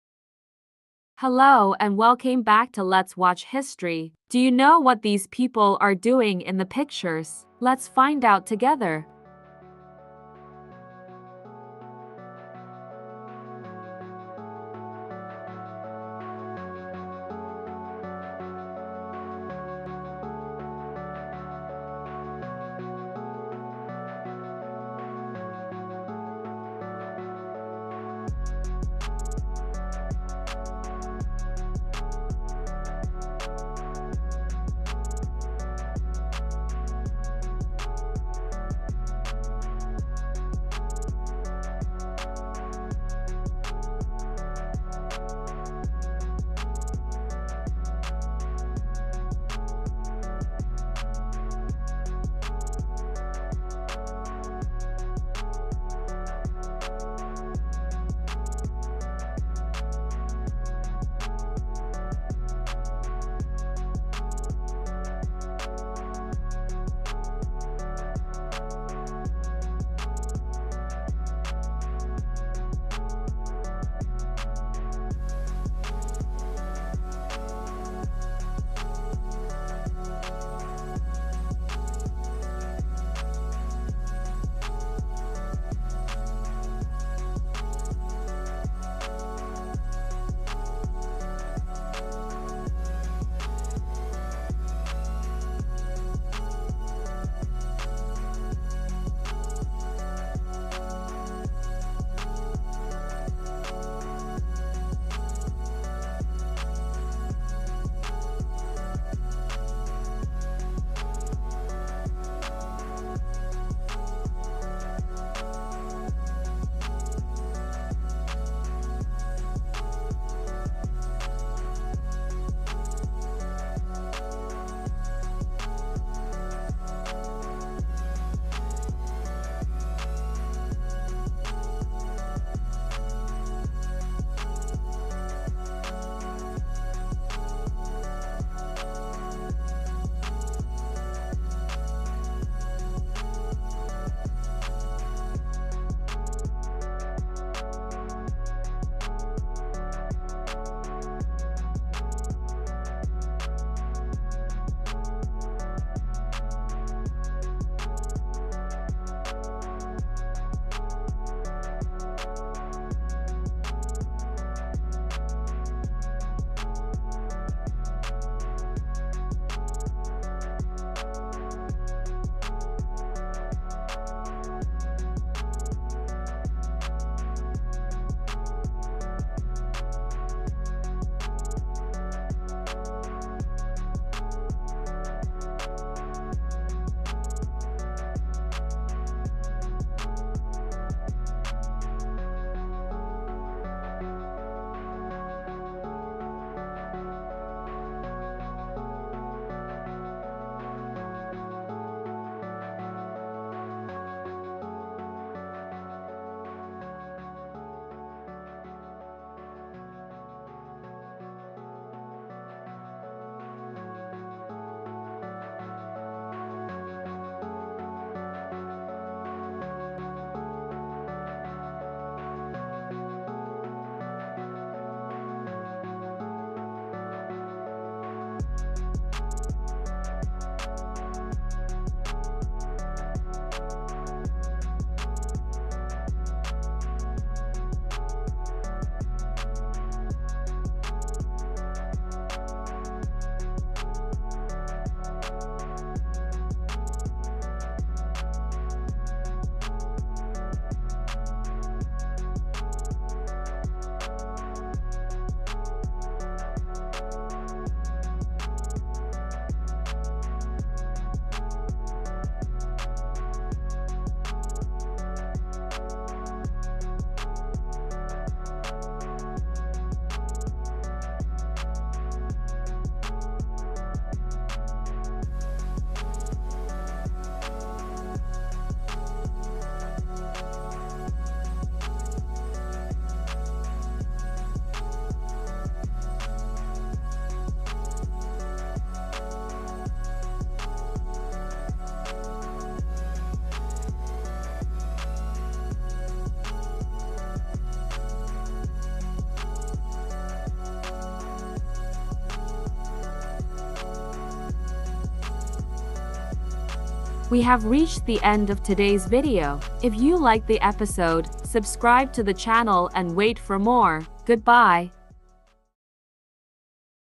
Find out why these guys climb trees! 📸⏳
in this new video from LET'S WATCH HISTORY channel we will show you 21 AMAZING HISTORICAL PHOTOS that shine a NEW LIGHT on some HIDDEN PARTS of History! 📸💛